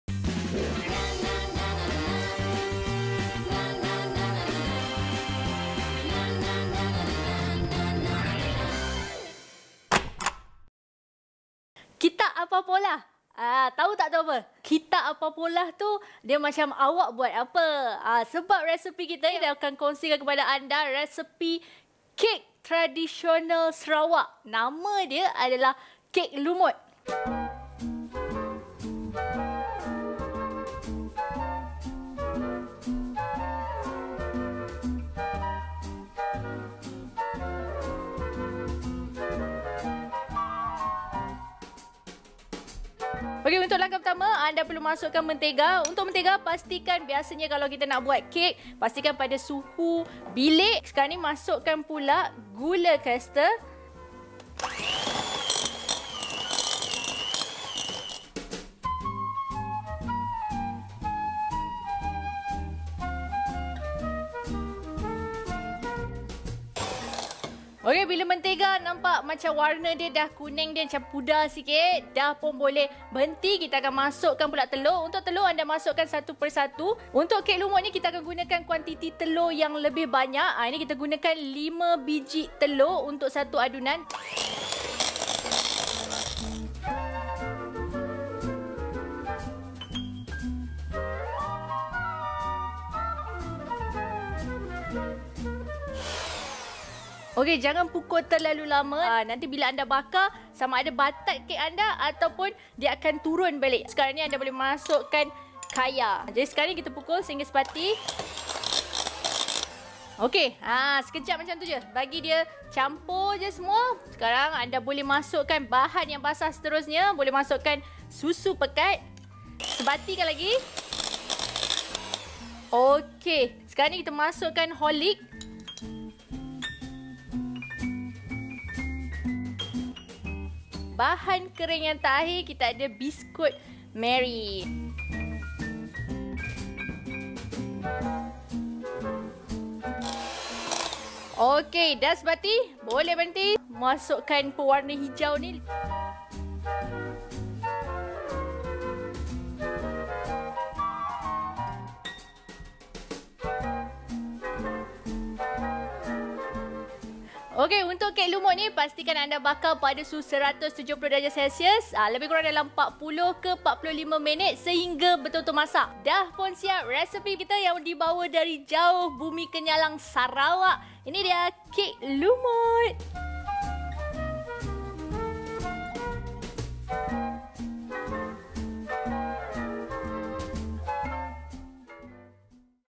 Bakar100%Kek - KEK LUMUT
Kek Lumut ni dah semakin dikenali kan? Korang ada yang tak pernah makan ke? Kalau korang nak buat mari tengok Heida Hidzir nak ajar cara buat Kek Lumut.